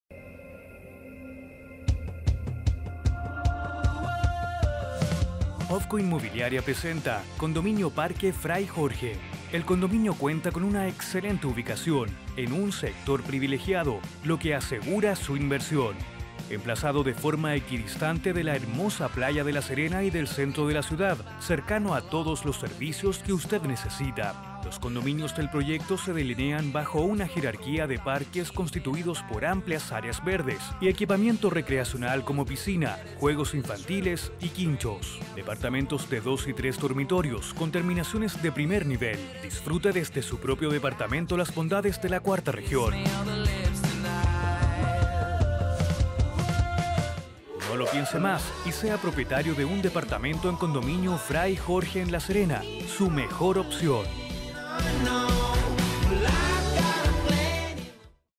PARQUE FRAY JORGE - OVCO
Tren Inmobiliario te presenta un condominio que cuenta con edificios de 6 pisos con, con una baja densidad de departamentos y construidos en líneas modernas y amplios espacios rodeados de jardines, en el eje cultural de La Serena.